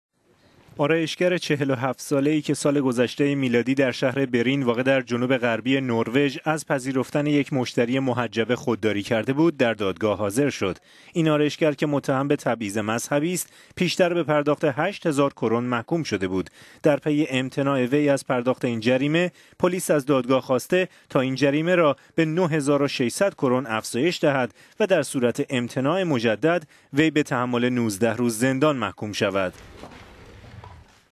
زن آرایشگر در نروژ به تبعیض مذهبی متهم شد
آرایشگر ۴۷ ساله ای که سال گذشته میلادی در شهر برین واقع در جنوب غربی نروژ از پذیرفتن یک مشتری محجبه خودداری کرده بود در دادگاه حاضر شد.

این زن آرایشگر که متهم به «تبعیض مذهبی» است پیش تر به پرداخت ۸ هزار کرون نروژ معادل ۸۷۰ یورو محکوم شده بود. در پی امتناع وی از پرداخت این جریمه، پلیس از دادگاه خواسته تا این جریمه را به ۹ هزار و ۶۰۰ کرون افزایش دهد و در صورت امتناع مجدد، وی به تحمل ۱۹ روز زندان محکوم شود.

متهم به تبعیض مذهبی در دفاعیات خود گفته است که «حجاب نمادی سیاسی است و نه مذهبی.» حکم داد…